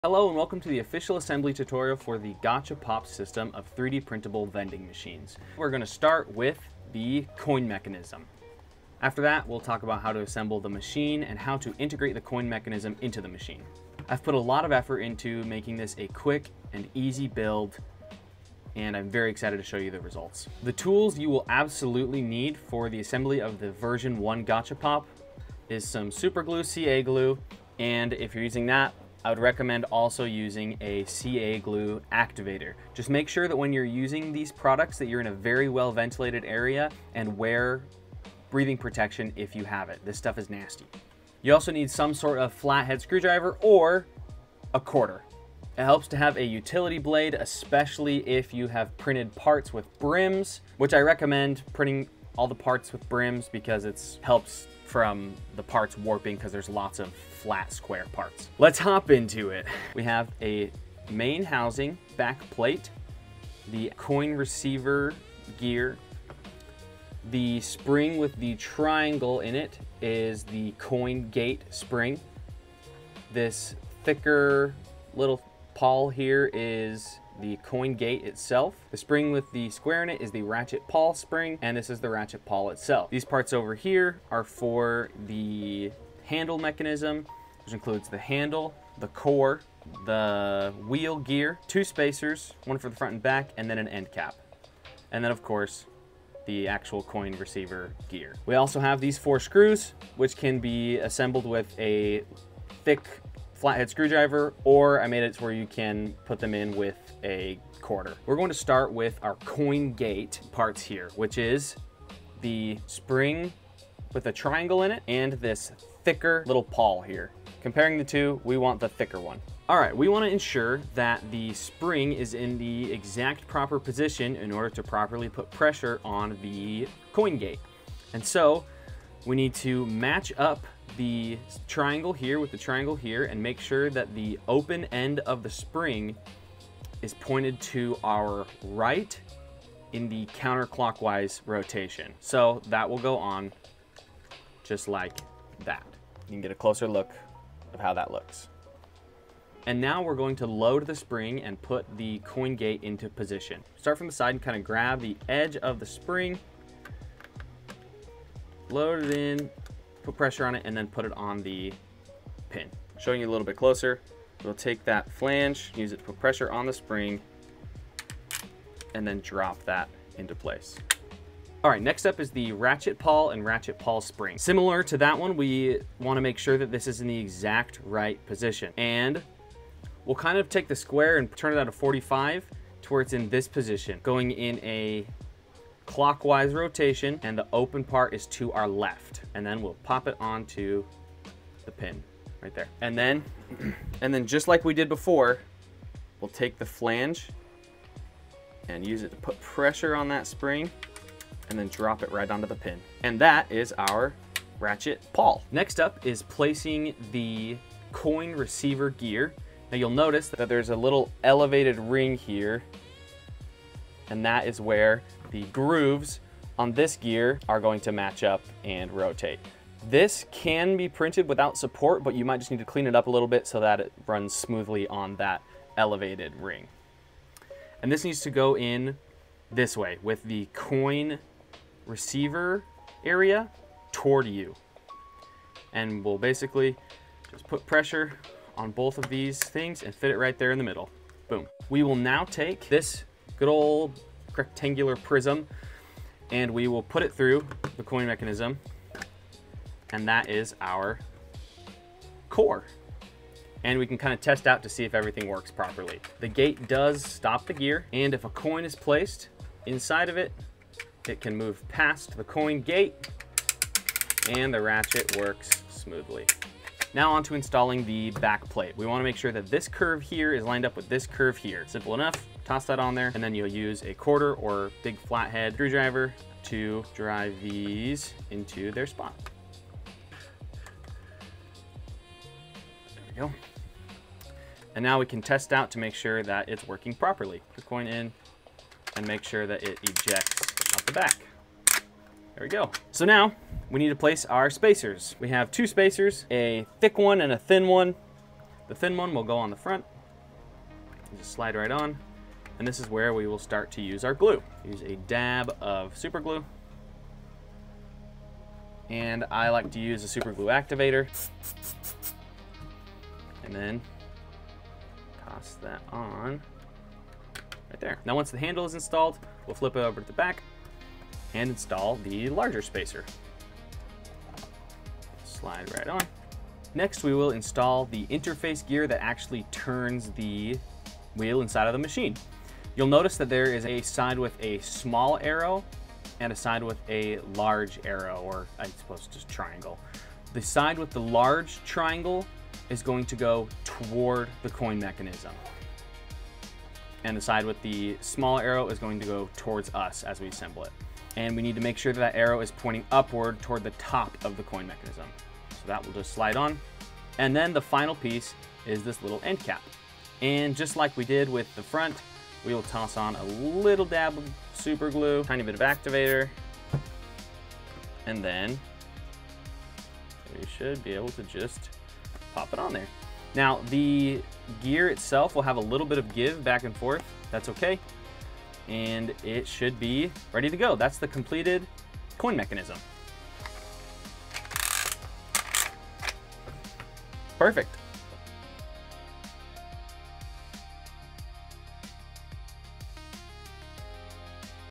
GachaPop Assembly Tutorial Pt. 1: Printed Coin Mech
Part 1 of the GachaPop V1 assembly guide! In this video, we are building the heart of the machine: the 100% 3D-Printed Coin Mechanism.

This mechanism requires zero metal hardware—just printed parts and a little bit of glue!

📂 WHERE TO GET THE FILES

(Includes the V1 Files + FREE Upgrade to the V2 "Zero-Glue" version!)

⚠️ IMPORTANT NOTE: V1 vs. V2
This tutorial is for the V1 "Early Access" model, which requires Super Glue or 3D Gloop for assembly.

If you purchased the Early Access pack, you will receive the V2 "Zero-Glue" version as a FREE digital update as soon as it is released!

🛠️ TOOLS & MATERIALS NEEDED FOR PT. 1
Printed Parts: Coin Mech

Adhesive: Super Glue (CA Glue) or 3D Gloop.

Time: Approx. 6 minutes